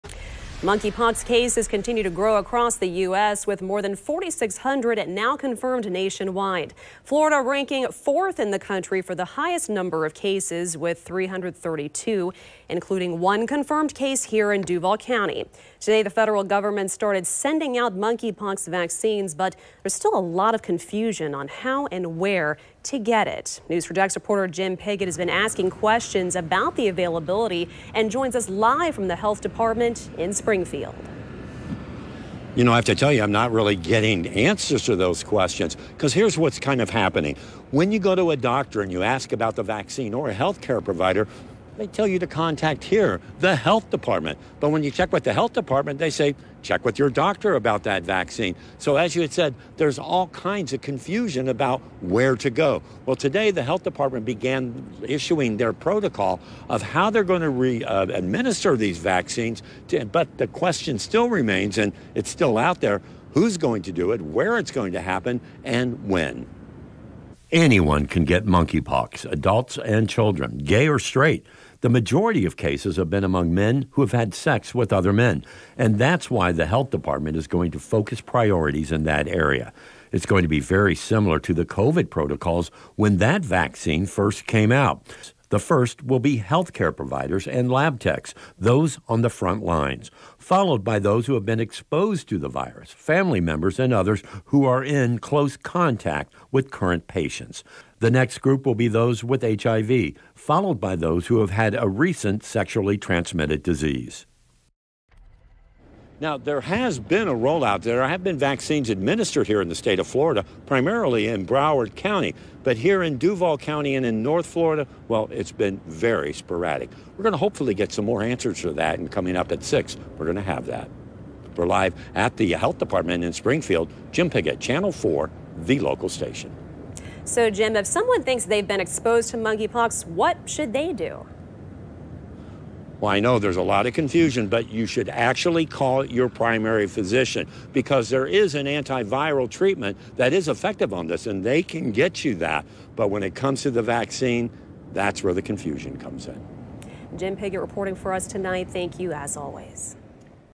Looking for monkeypox vaccine in Northeast Florida? Here’s what you need to know
As the federal government prepares to send out 800,000 doses of the monkeypox vaccine, many are still confused about how and where to get it when it comes to their town.